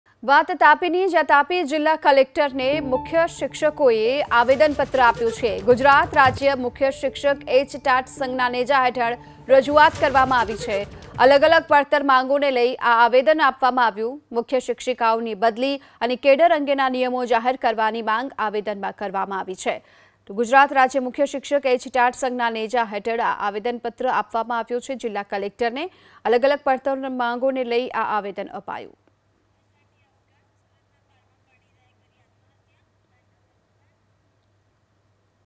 Tapi News : જિલ્લા કલેક્ટરને મુખ્ય શિક્ષકોએ આપ્યું આવેદન, જાણો શું છે સમગ્ર મામલો
Tapi | Tapi News | Collector | Teachers | Avedanpatra